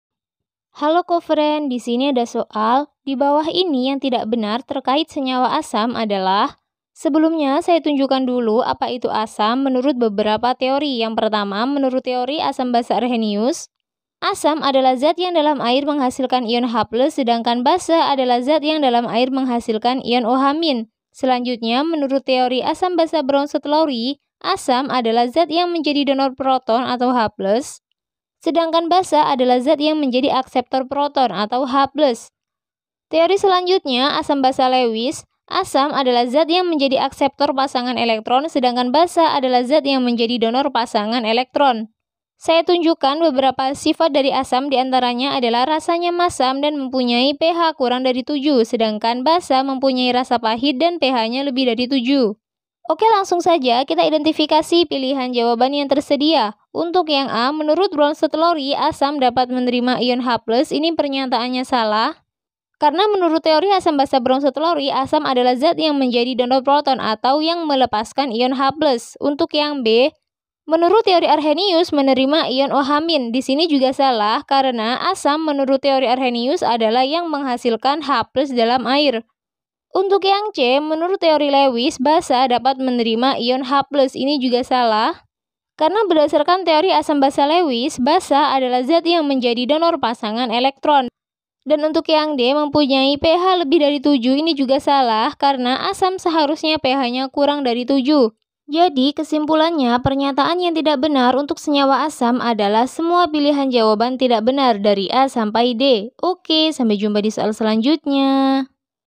Dibawah ini yang tidak benar terkait senyawa asam adalah menurut teori Bronsted Lowry, asam dapat...
Dibawah ini yang tidak benar terkait senyawa asam adalah menurut teori Bronsted Lowry, asam dapat menerima ion Ht a) b) menurut teori Arrhenius menerima ion OH" menurut teori Lewis, basa dapat menerima ion Ht C) d) mempunyai pH &gt; 7

Topik atau Materi: pH Asam Kuat, Basa Kuat, Asam Lemah, dan Basa Lemah - Asam dan Basa - Kimia Fisik dan Analisis - Kimia Kelas 11 SMA -

Sumber Soal: Kimia Quizizz Kelas 11 , N/A

00:00 : Bedah Soal
01:00 : Konsep, rumus dan pengertian pH Asam Kuat, Basa Kuat, Asam Lemah, dan Basa Lemah
01:18 : Langkah penyelesaian soal
00:49 : Jawaban akhir
01:19 : Penutup